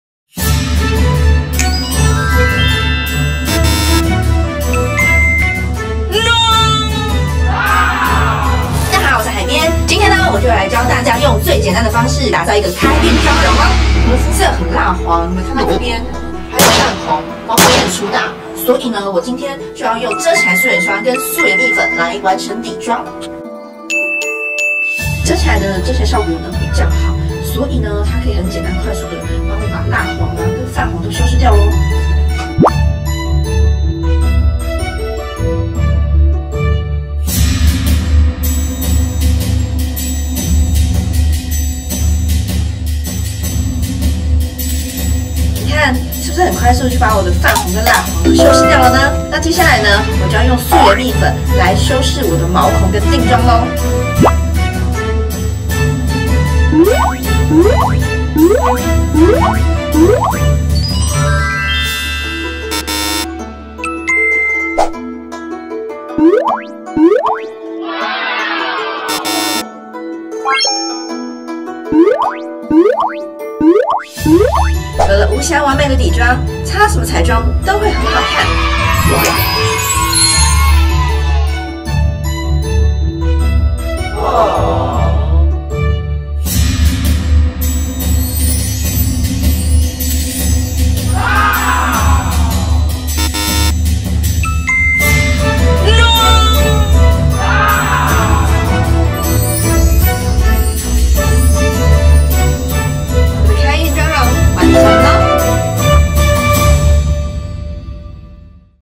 【MKUP美咖】輕薄遮瑕零妝感＄６４０／入
量膚訂製最美素顏❤️簡單開運彩妝往下看
遮瑕保濕持效、SPF23★★★紫外線剋星
一拍柔焦毛孔、快速上妝
肌膚質感瞬間升級😍不敗對不起自己！

💦加倍潤色，妝感更自然
💦舒緩肌膚，溫和配方
💦新一代水潤科技，保濕升級

🔹一拍柔焦毛孔
🔹粉質細緻，清爽薄透
🔹親膚配方，溫和不易刺激
MKUP分享王社團不可錯過！

※因為每個人膚色、膚況不同，產品效果使用起來會不一樣唷！